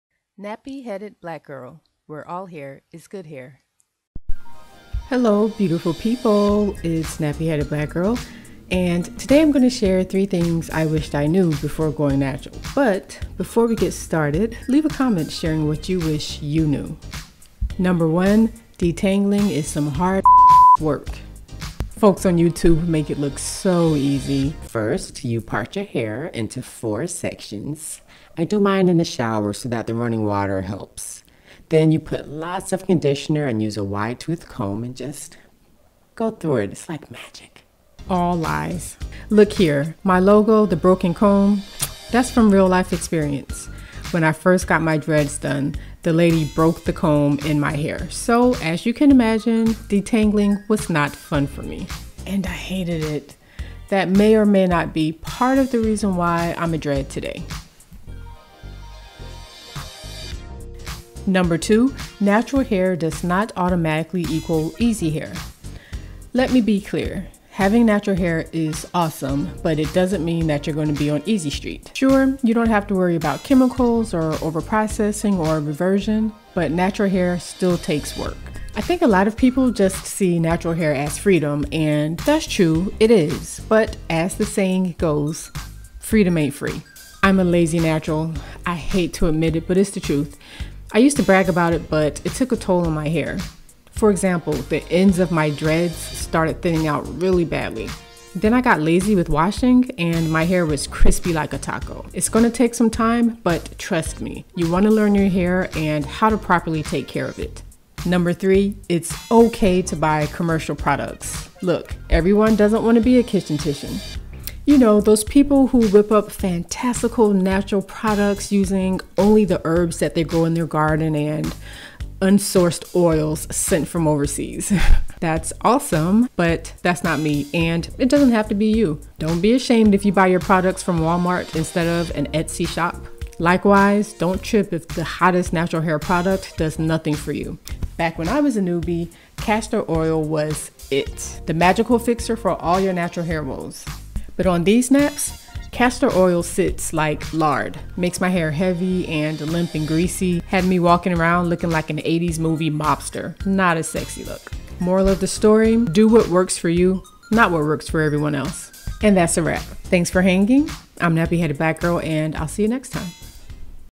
3 Things I Wish I Knew Before Going Natural 💡
3 things I wish I knew before going natural. Watch this video before YOU go natural 👍